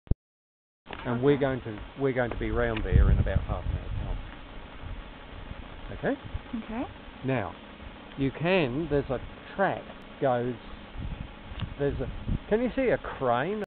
Hillgrove Mines NSW
A very quick view from the top of the hills of Hillgrove.